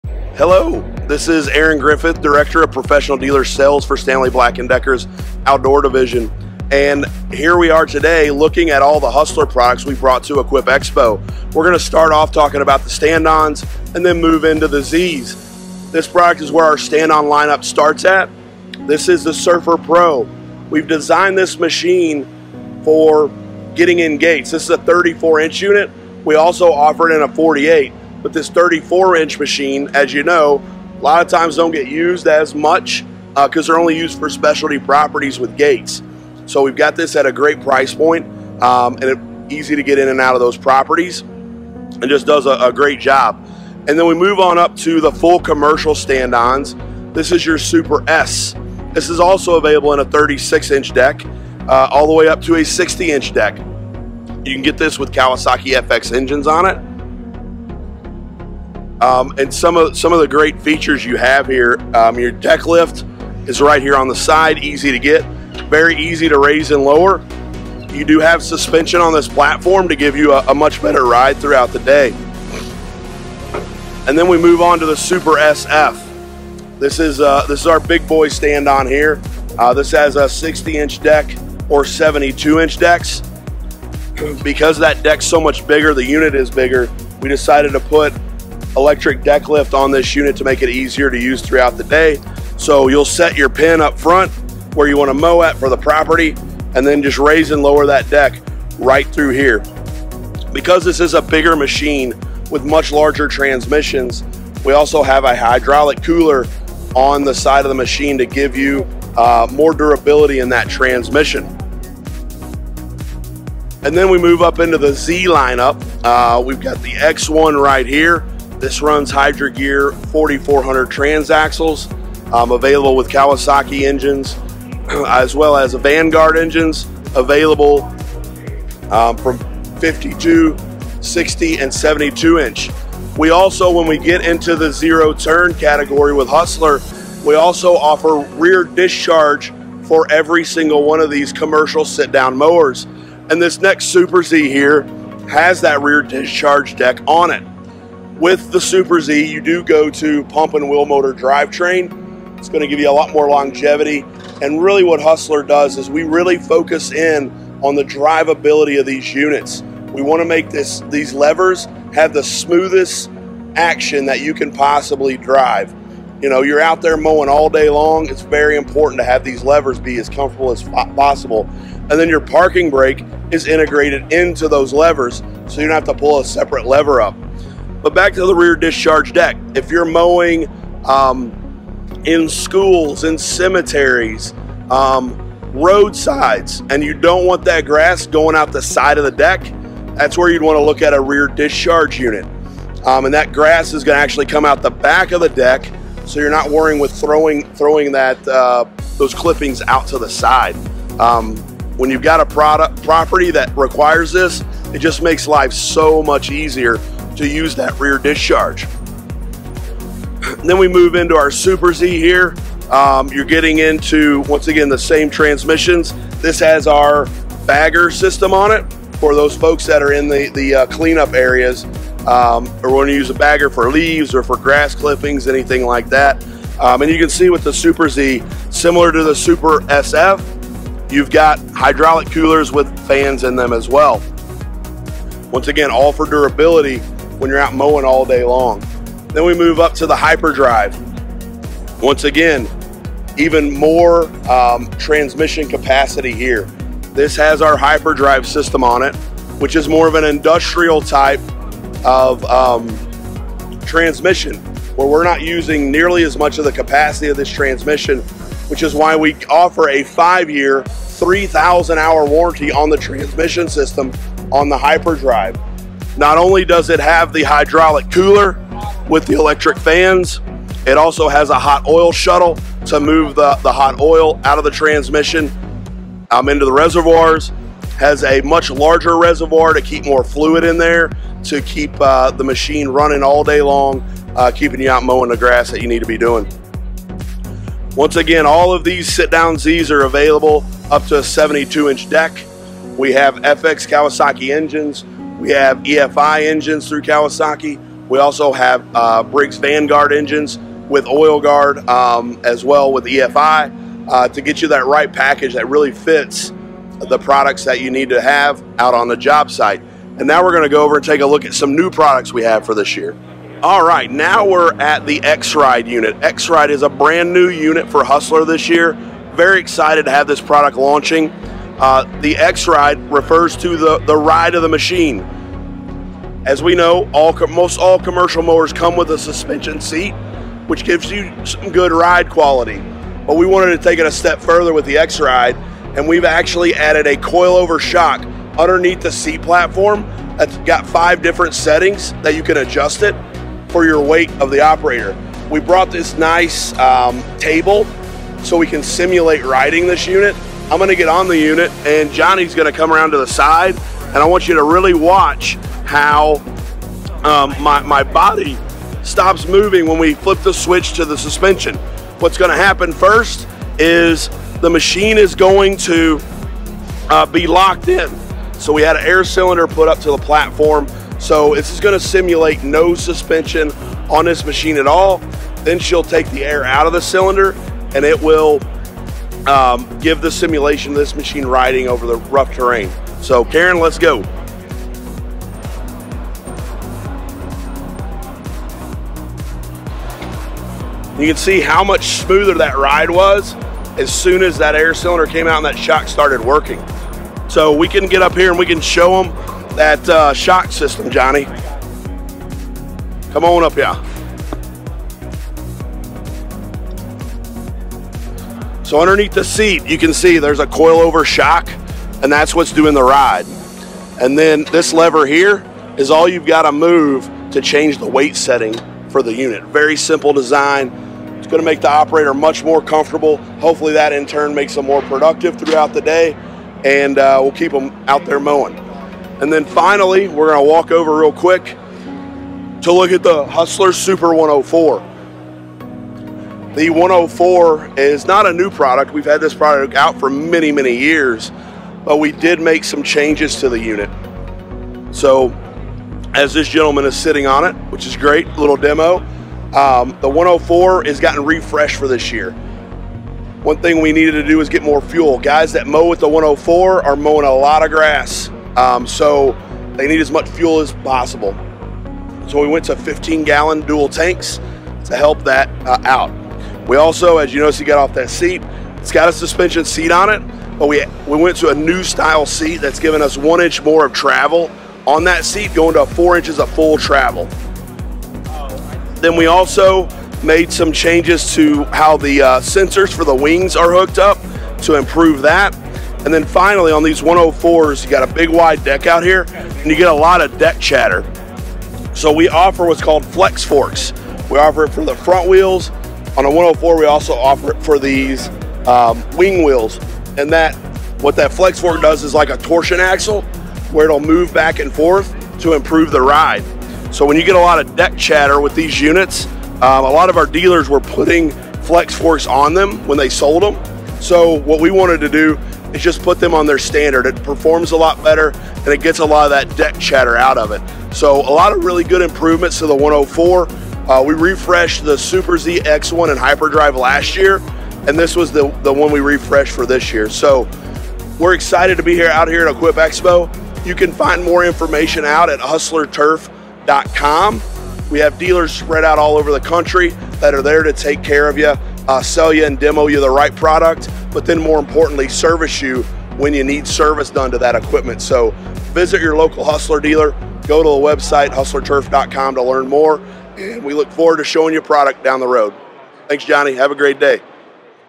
HUSTLER MOWERS 2024 LINE UP
use BLADES10 as your password!

Motorized Chute blocker "BLADES10" $10 OFF!

00:00-00:16 Intro
00:17-00:44 HUSTLER Surfer PRO
00:45-01:15 HUSTLER Super S
01:16-01:52 HUSTLER Super SF
01:53-02:23 HUSTLER X-ONE
02:24-03:29 HUSTLER SUPER Z
03:30-03:58 HUSTLER SUPER Z with Bagger
03:59-05:12 HUSTLER HYPERDRIVE
05:13-07:16 HUSTLER X-RIDE DEMO
07:20-09:18 HUSTLER SUPER 104